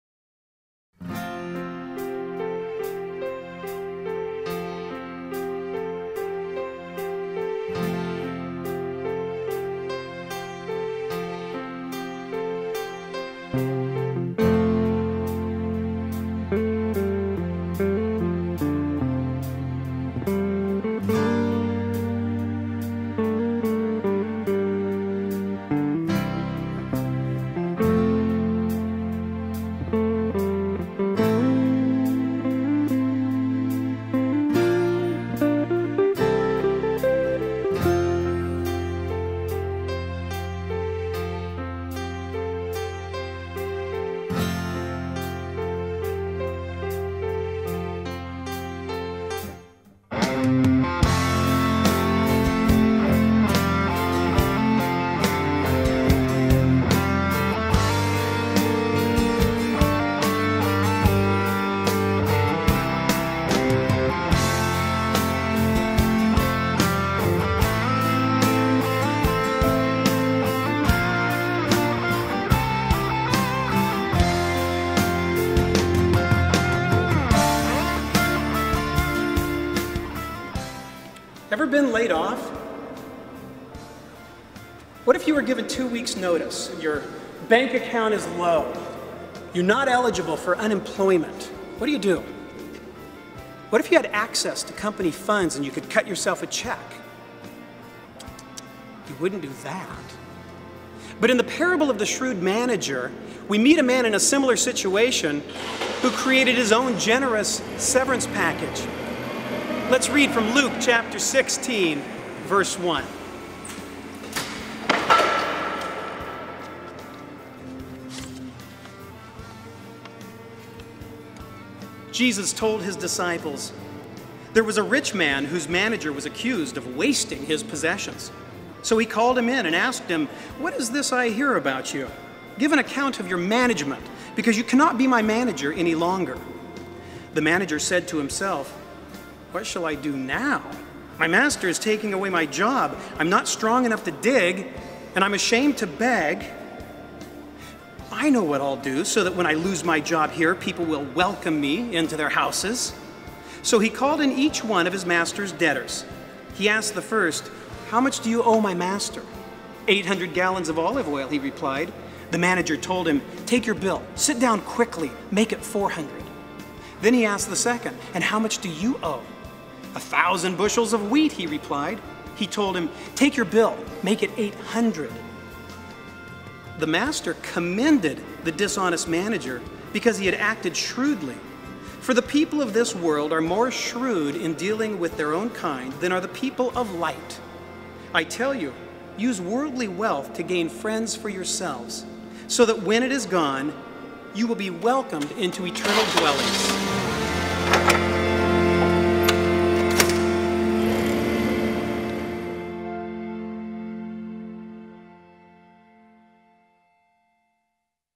The Shrewd Manager
If our love for God isn't greater than our love of money, we won't be able to be spiritually shrewd with our worldly wealth for His eternal purpose.